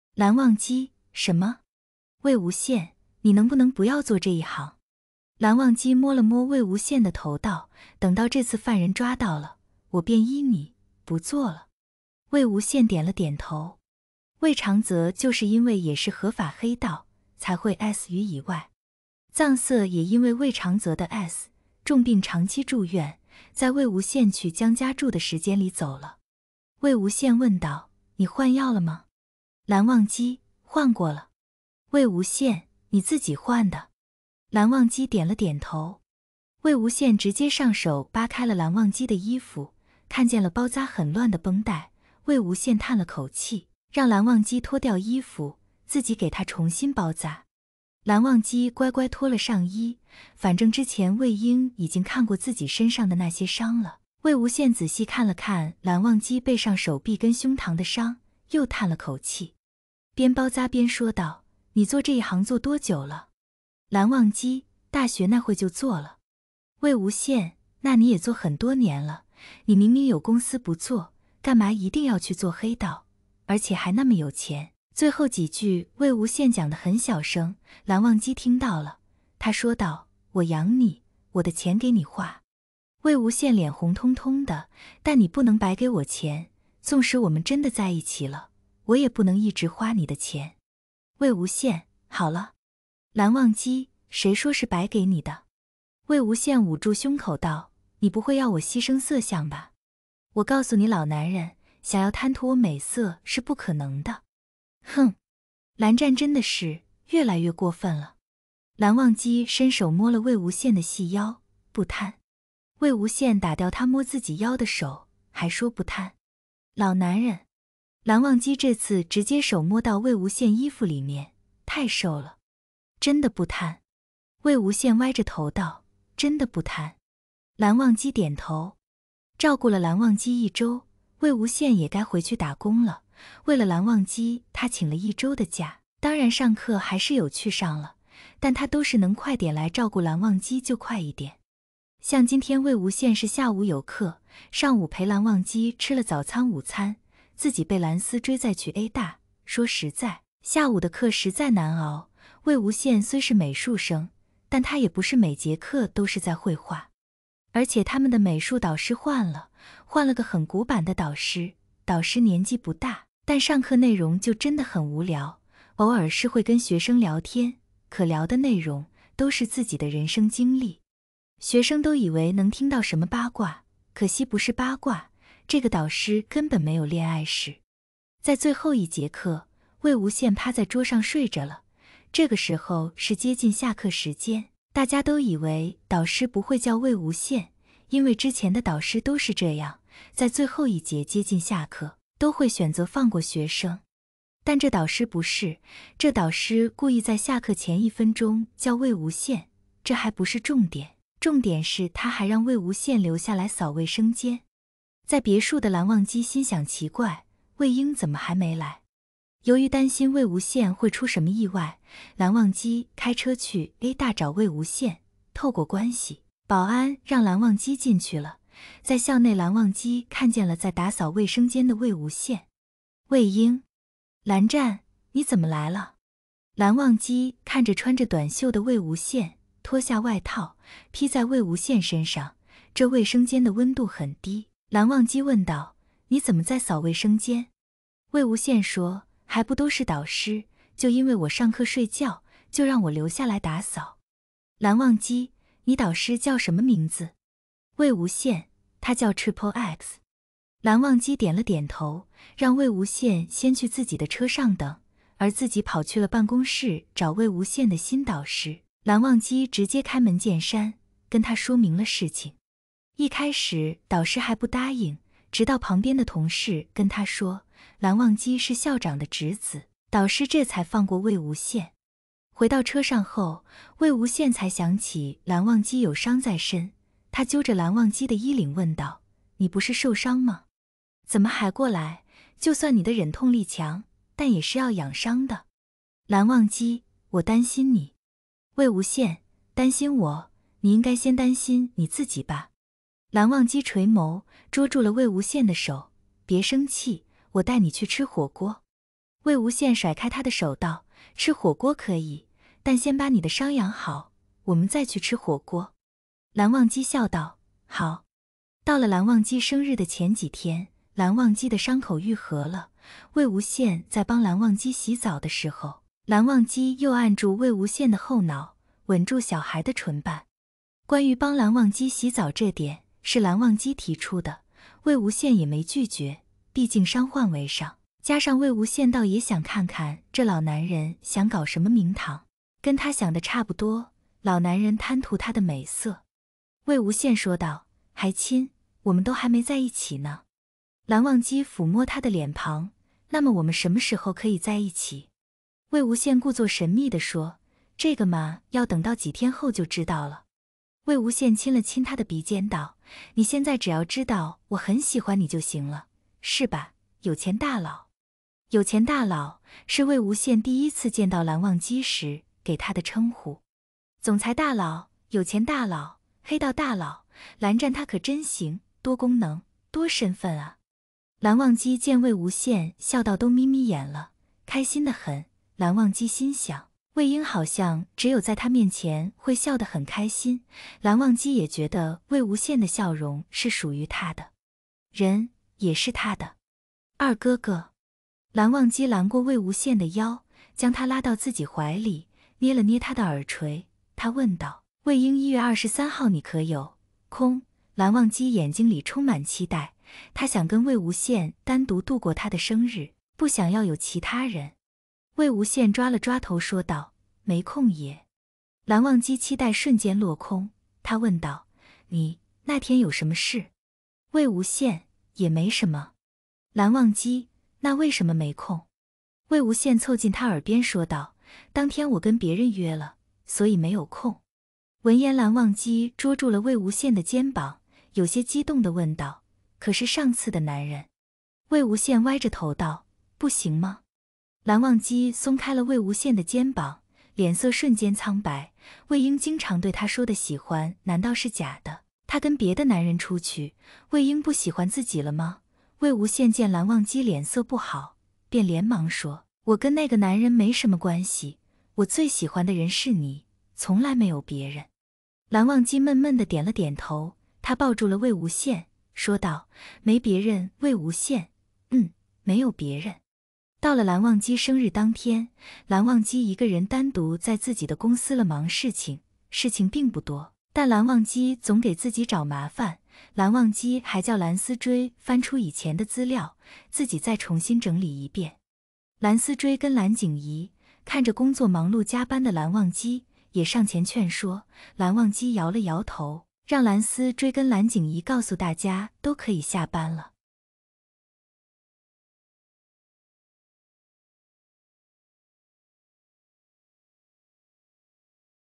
忘羡《追纯情十九岁魏无羡》第20集魏無羨:“擔心我?你應該先擔心你自己吧!”藍忘機垂眸，捉住了魏無羨的手:“別生氣，我帶你去吃火鍋?"魏無羨甩開他的手道:“吃火鍋可以，但先把你的傷養好!我們再去吃火鍋
温柔腹黑叽×纯情可爱羡现pa+甜文#魔道祖师#忘羡